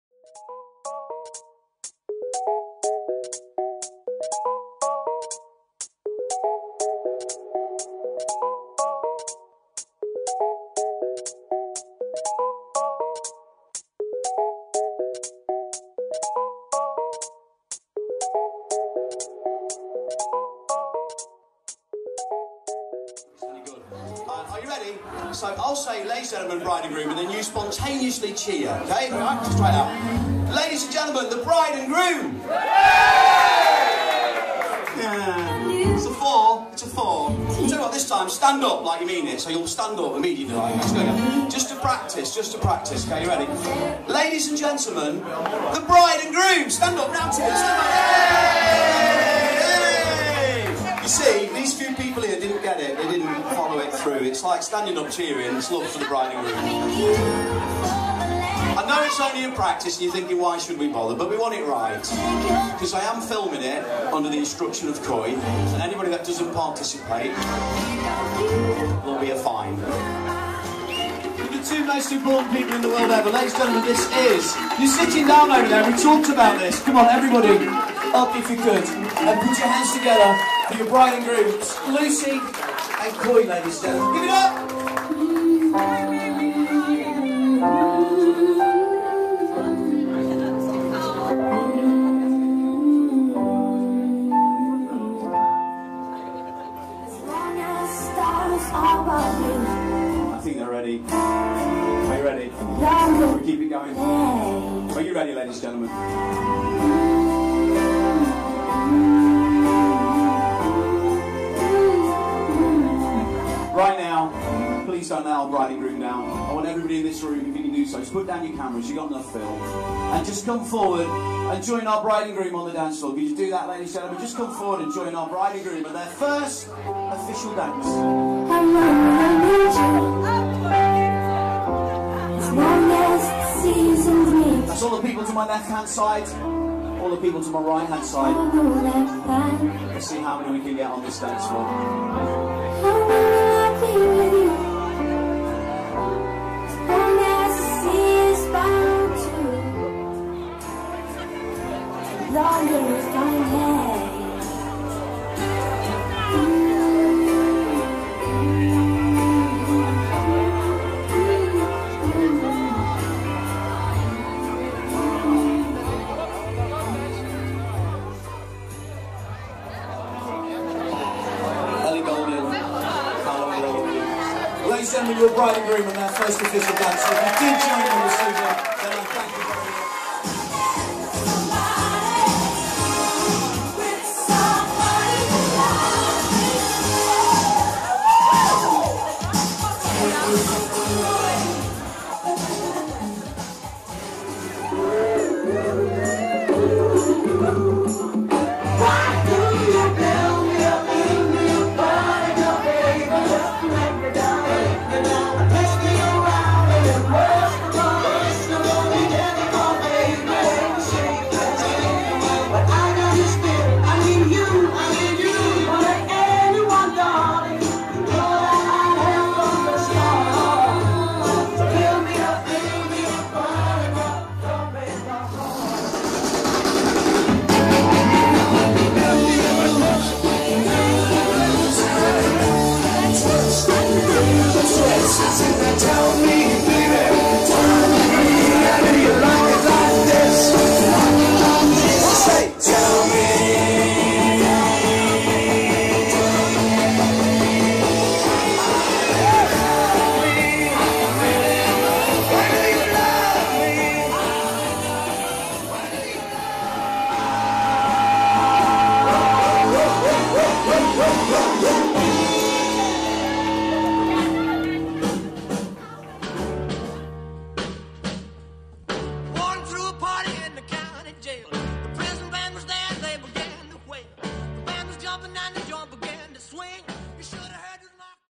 Lucy & Koi's Wedding @ Lea Marston Sutton Coldfield B76 0BY (1)
Dave Dee Discos have entertained on a number of occasions before at Lea Marston, but to date never the Perry Barton Suite, so it was vital we got to see venue before the event  to familiarise ourselves with the room and best advise on where the disco should be placed and what we could do to "Light" and enhance the room. This I feel we did with ease, lighting the dance floor the walls and the roof using a mix of static and vibrant moving beams of light and colour.